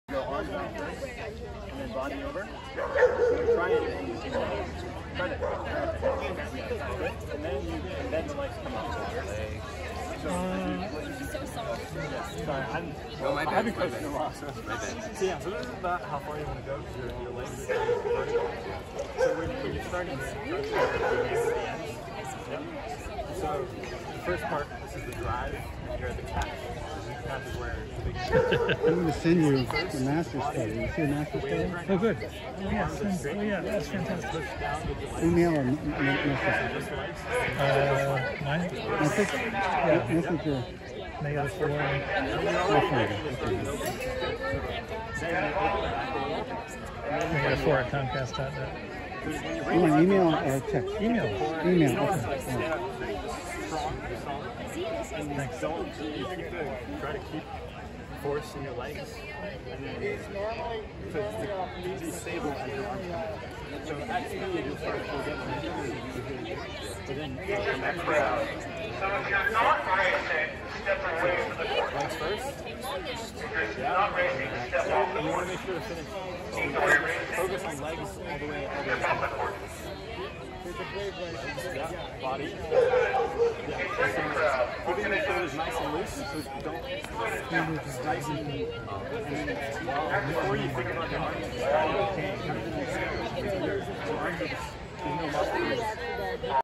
Wayland-Weston Crew 2021 Ergathon & Pancake Breakfast 12
Wayland-Weston Crew ergathon and pancake breakfast September 25, 2021.